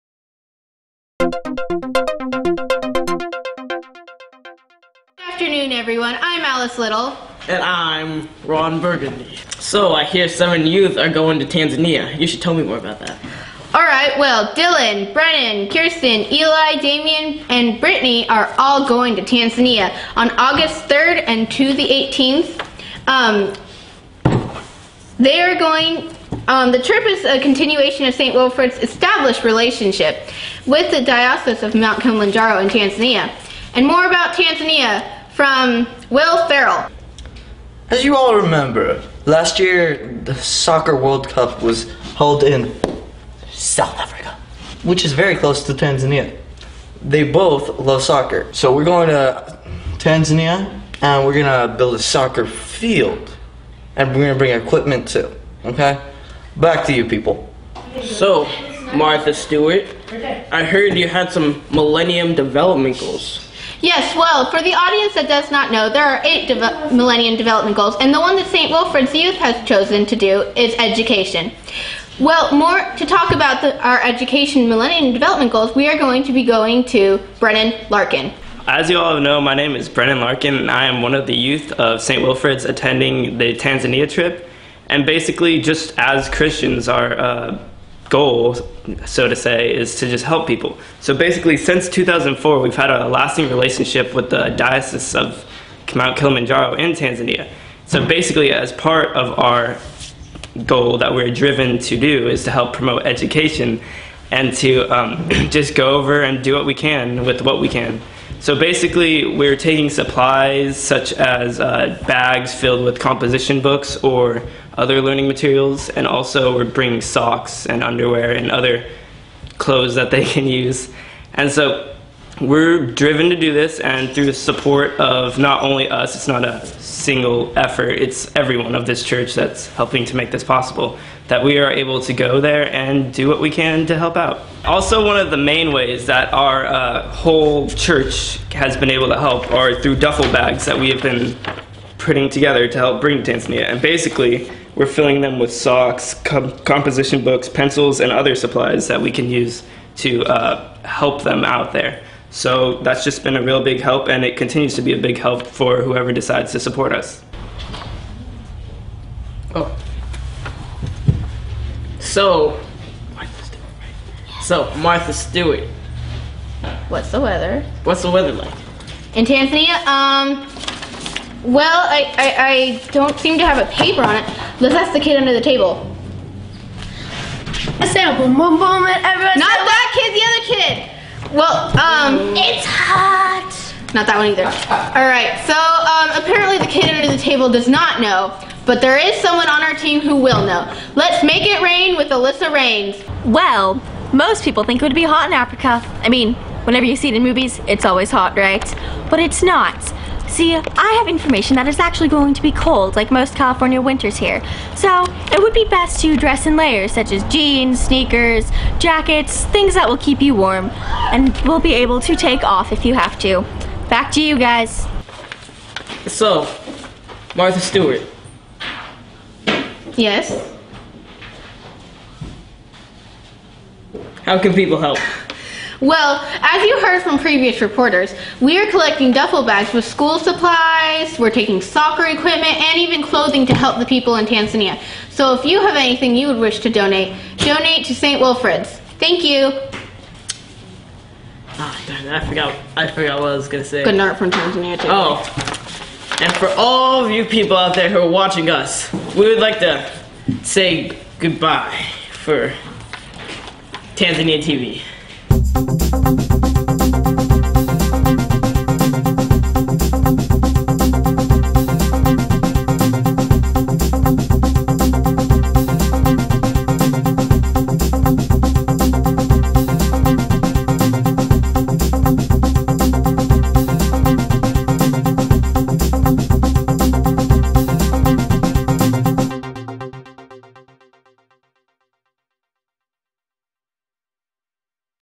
Tanzania Tea V
This is the official news reel for the youth's trip to tanzania in August 2011! In this video, you will learn about the youth's goals in tanzania and what we will be doing back here to help too!